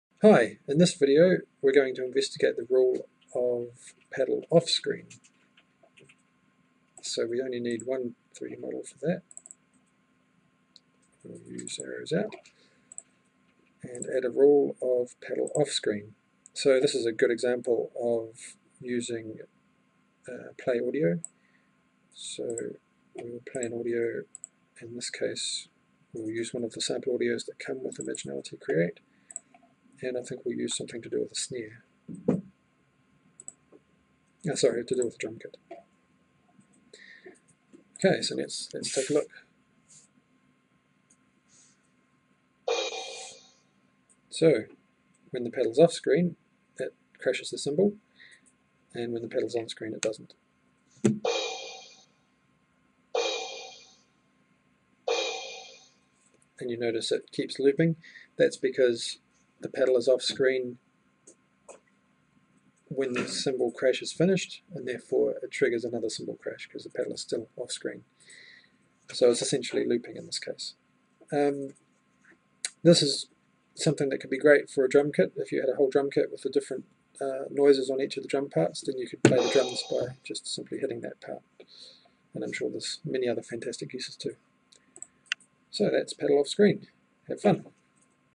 Imginality Create Help - Rule - Trigger - Paddle off Screen
This video shows how to use a Rule Trigger of "Paddle off Screen" in Imaginality Create This includes and example of playing a sound every time a paddle is hidden.
You can also download the module created in this video to run it and modify it